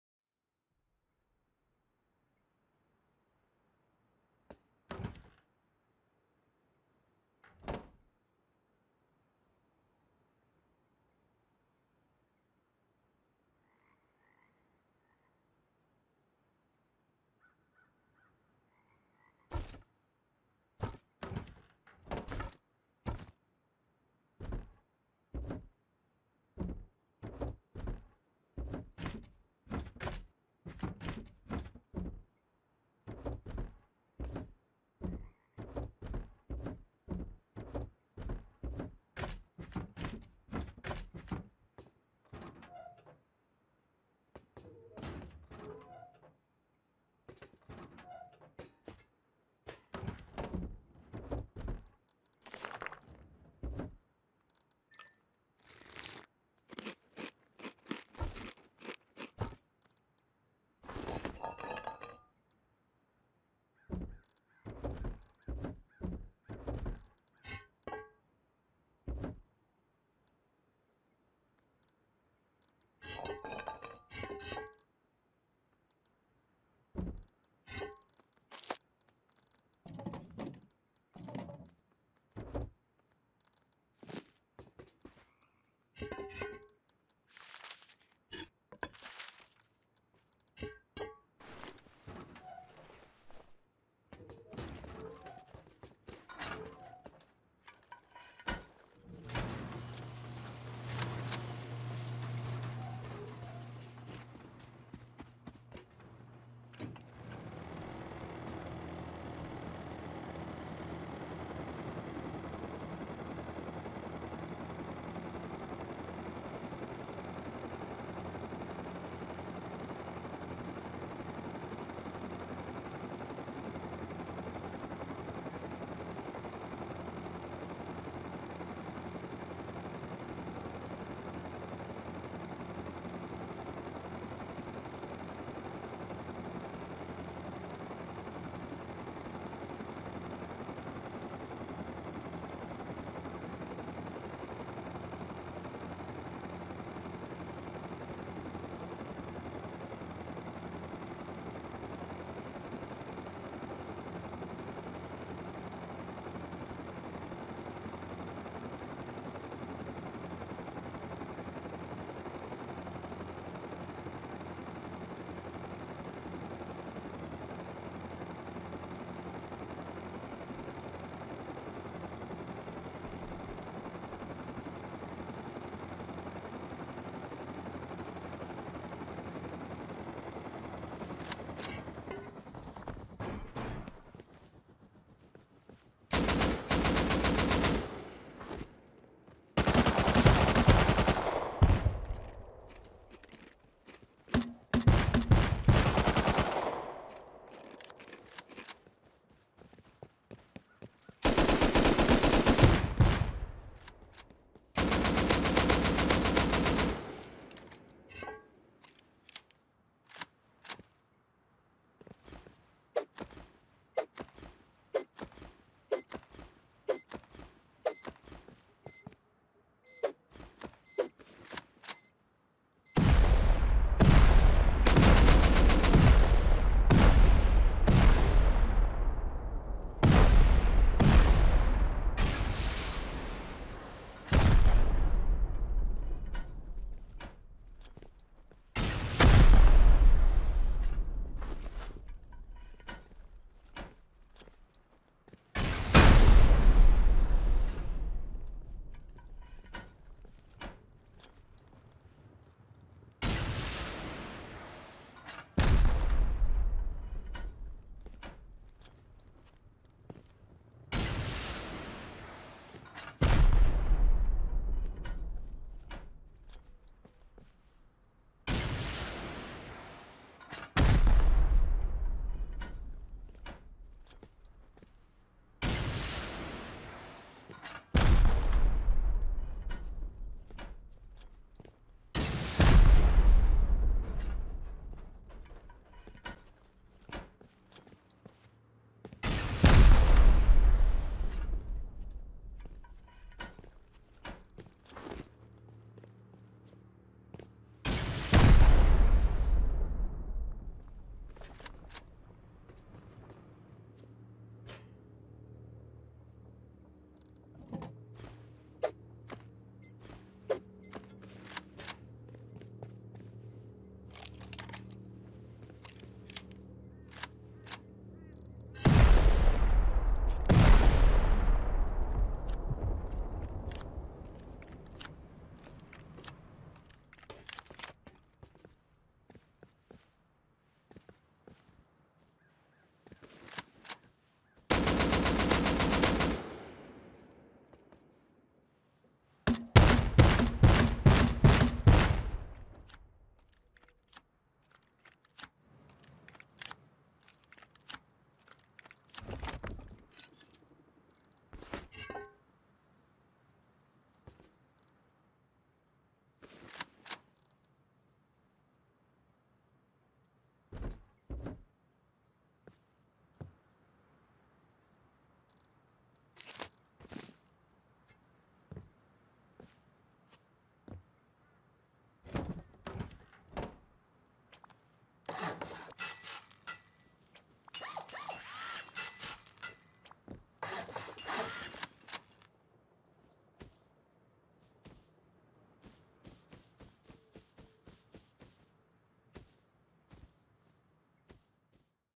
extremely fast online raid
last day of wipe. yolo'd toward explosions in the distance with a ton of boom hoping for an epic base to hit. server is a 3x if ur wondering.